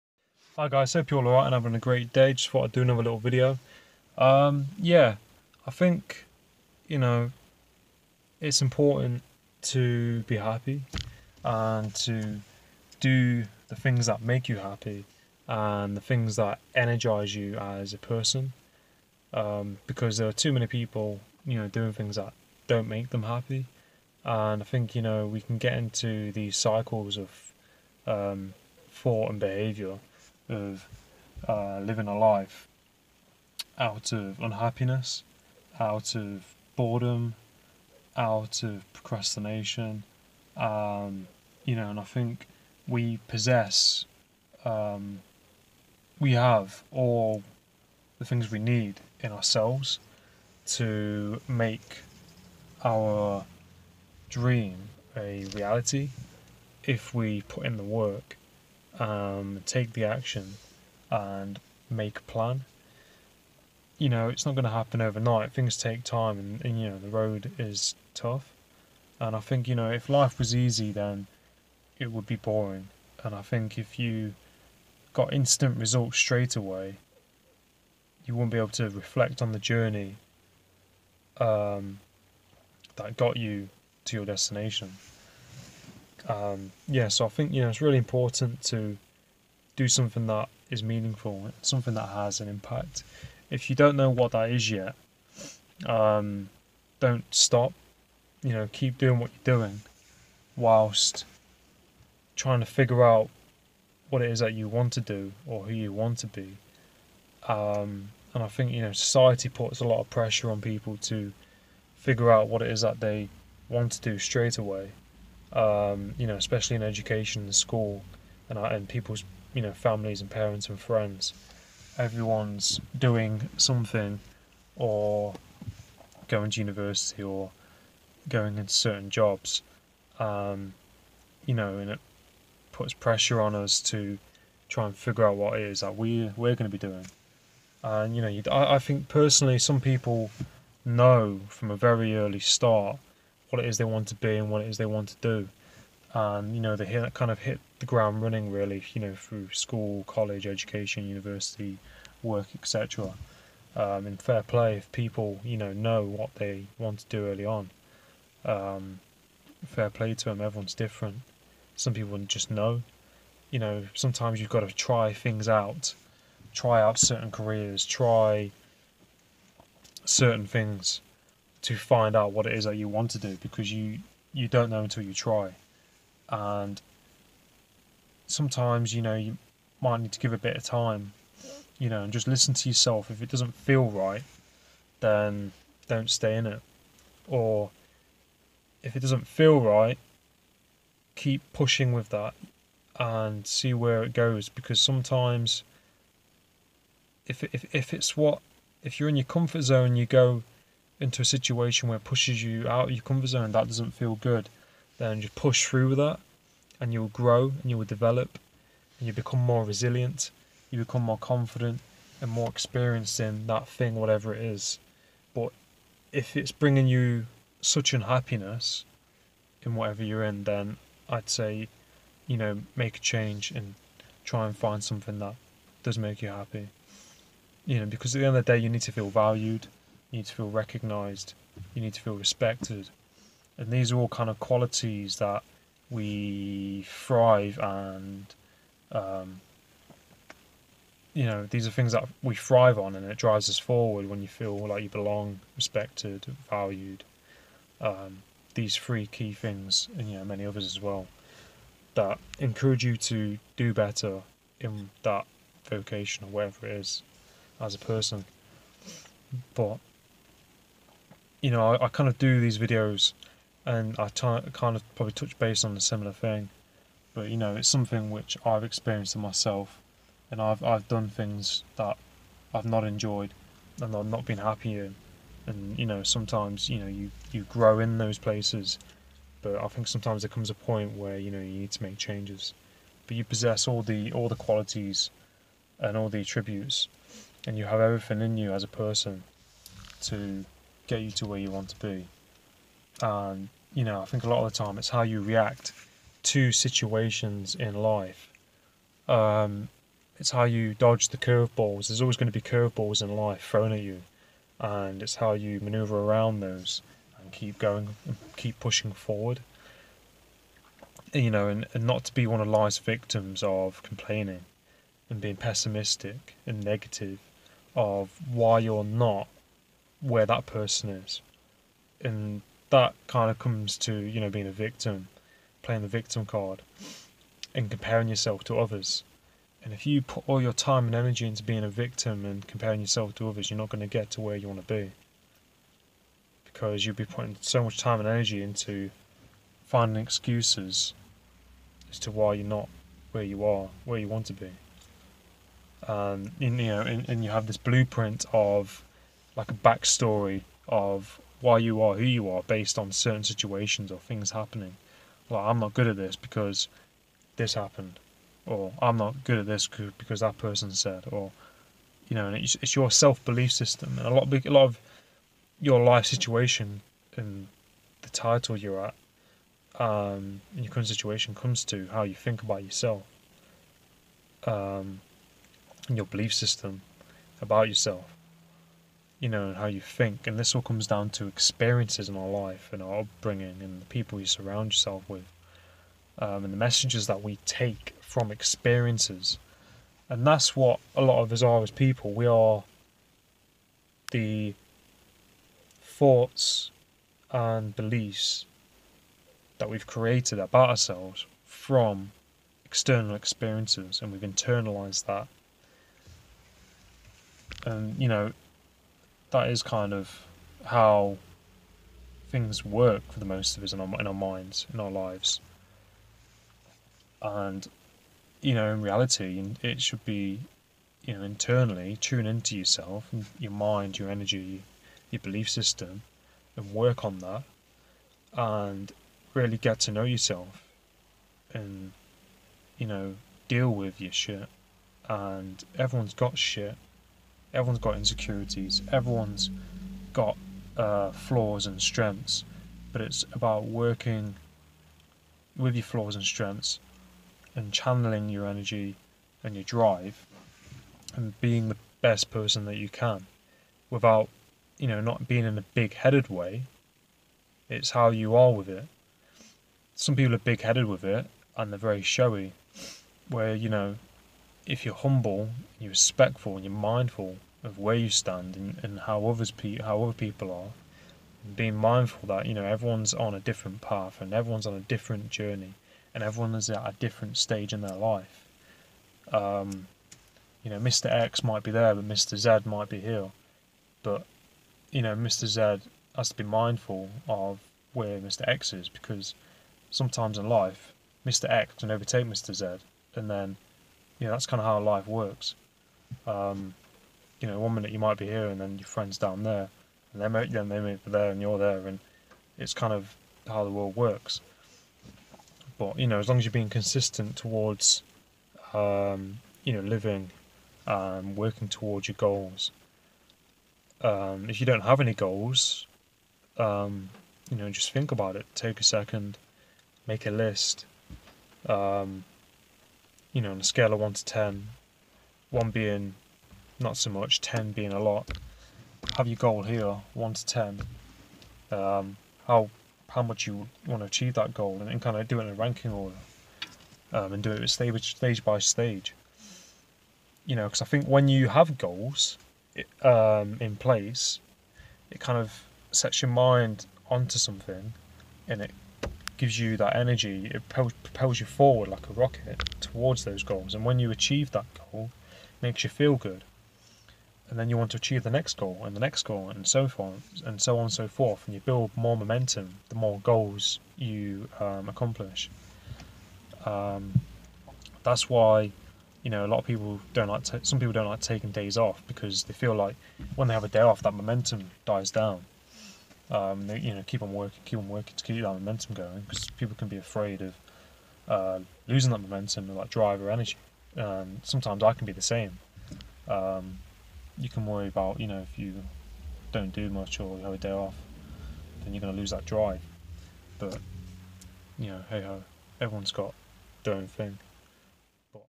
How to life a better more effective life
Here is a video that expresses on my opinions and thoughts on what it takes to live a happy life, and some key things that i feel are important.